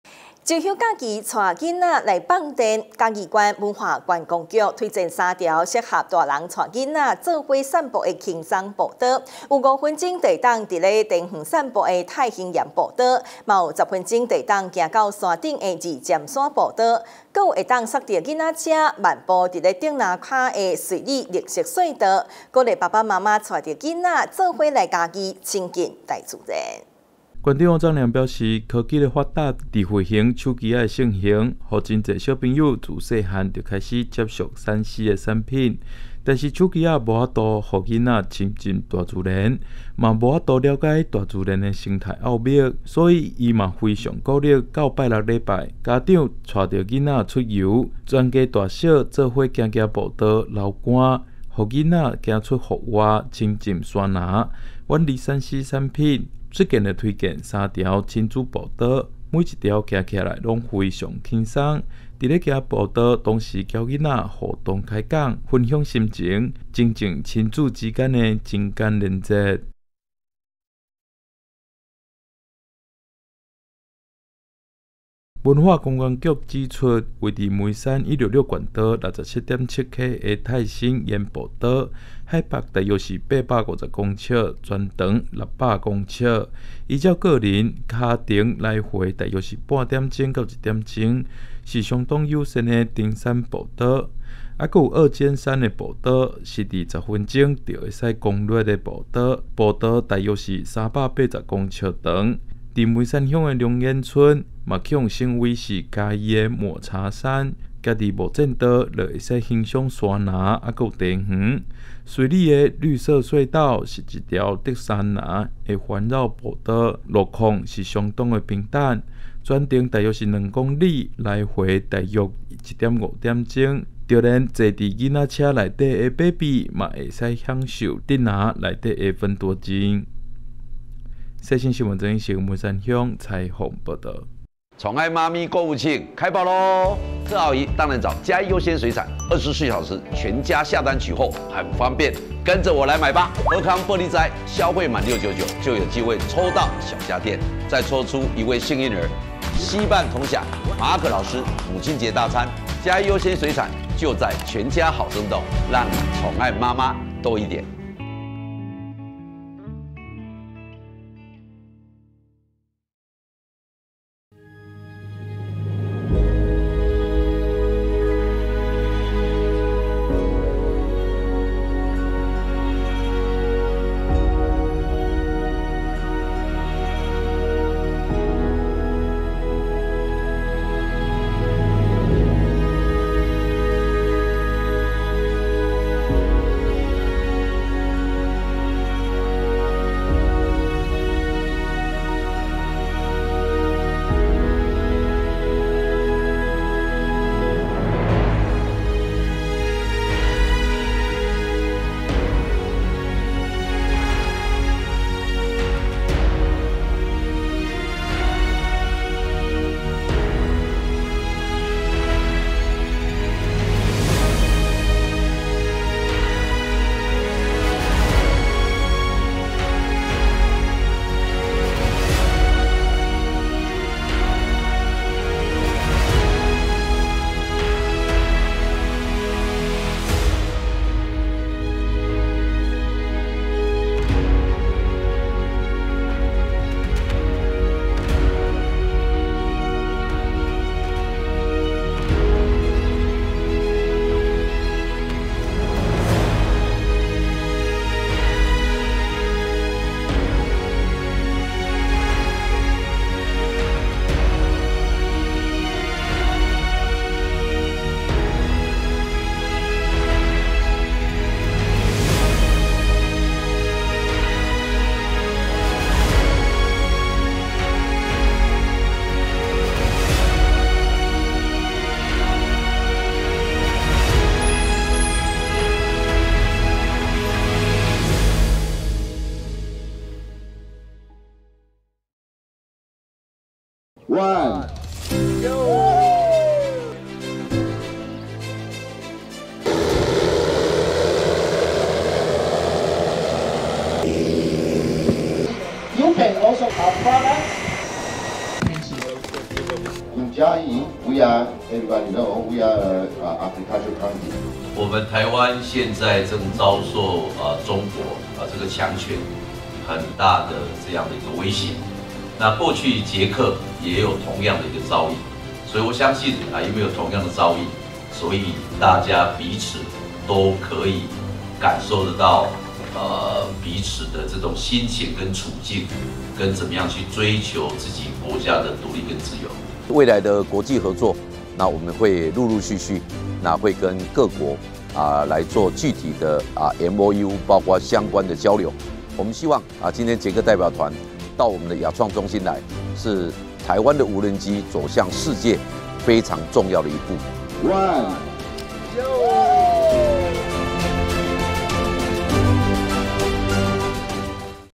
世新新聞 親子淺山小旅行 嘉義推薦3條幼幼級步道
112年4月19日 (三) 世新新聞

親子淺山小旅行 嘉義推薦3條幼幼級步道

周末帶小孩放電去！嘉義縣文化觀光局推薦3條適合親子的超輕鬆步道，有五分鐘就能漫步茶園的太興岩步道，有10分鐘就能攻頂的二尖山步道，還有可以推著娃娃車漫步在竹林下的瑞里綠色隧道，以最不耗費體力的方式呼吸新鮮空氣，享受芬多精欣賞山嵐美景，讓爸爸媽媽帶著孩子們親近大自然一點也不困難。